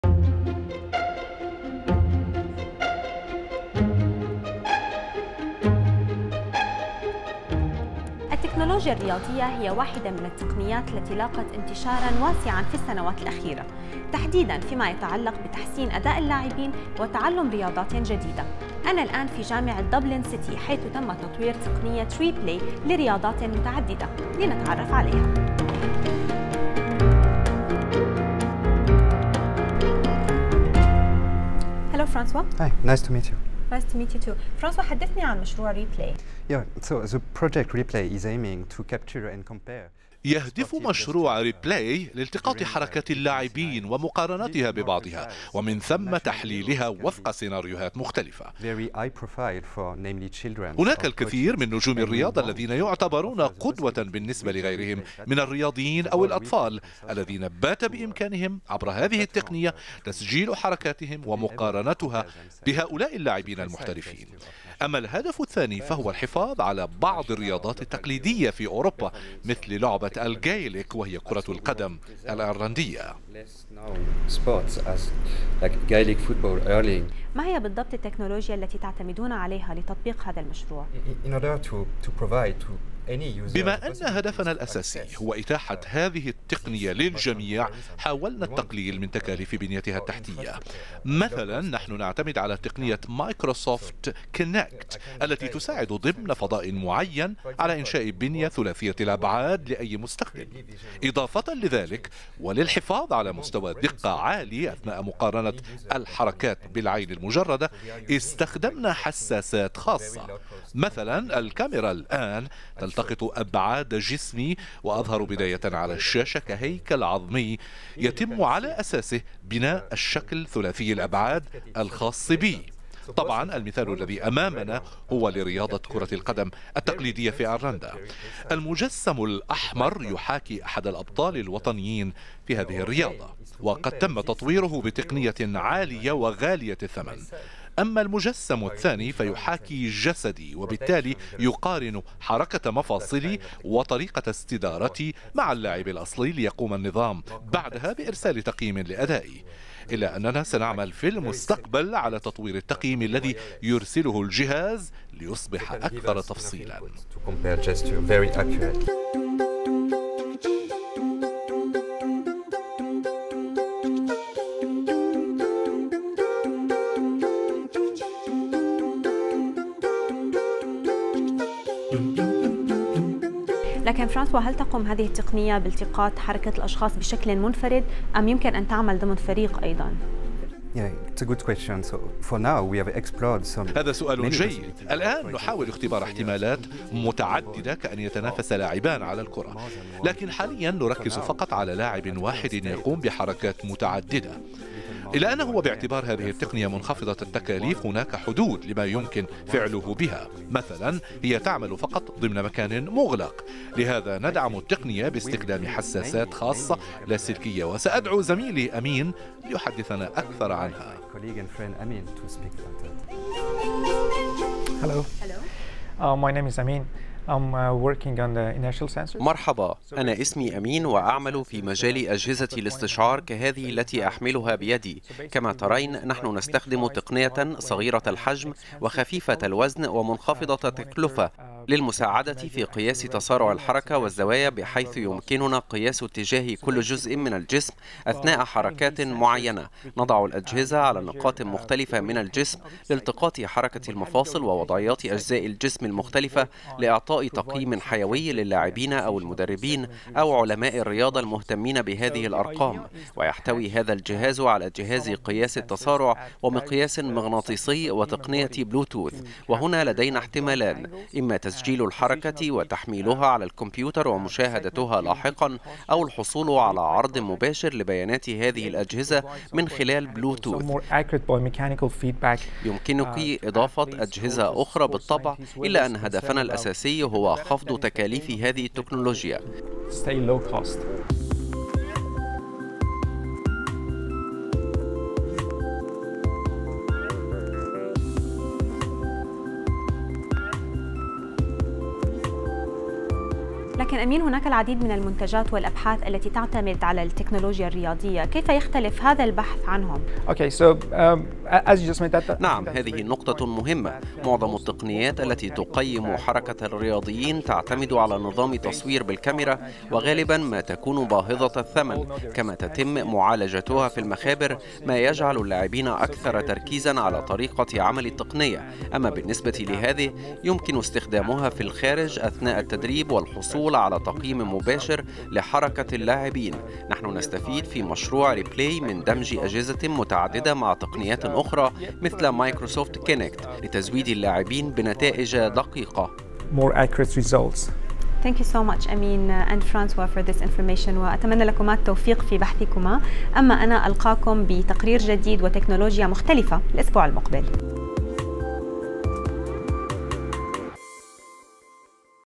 4تك: تقنية Reply لتقييم اداء اللاعبين عقب التدريب
تشهد التقنيات الرياضية تطورا هائلا في السنوات الاخيرة ما ينعكس إيجابا على أداء اللاعبين. من جامعة مدينة دبلن تختبر داليا حيدر تقنية Reply  التي تعتمد التقنية بشكل اساسي على اجهزة استشعار تلتقط حركات اللاعبين ليتمكنوا من تقييم ادائهم عقب التدريب.